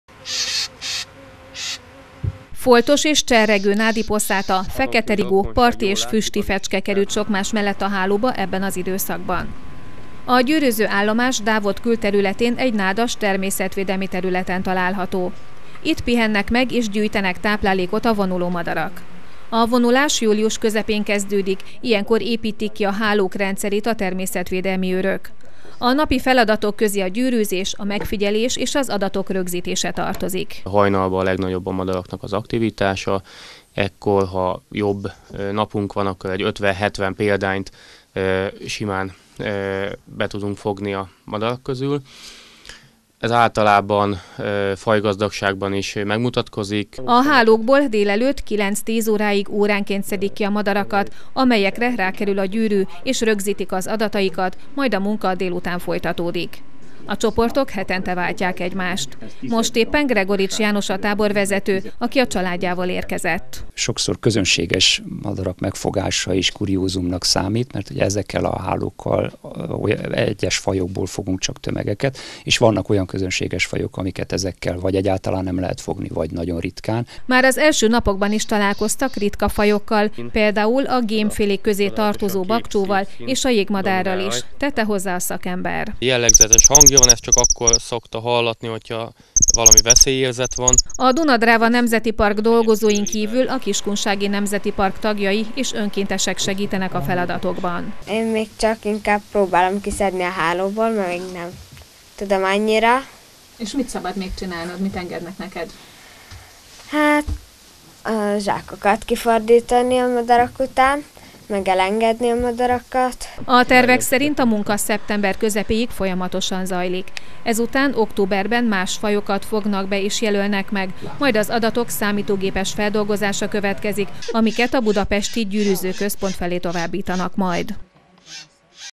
Madárgyűrűzés Dávodon
Hetek óta zajlik a munka a dávodi madárgyűrűző állomáson. Az egyes fajok megjelölése egész évben folyik, a vonulási időszakban azonban tömegesen jelölik meg a madarakat. A munkában szakemberek és önkéntesek vesznek részt.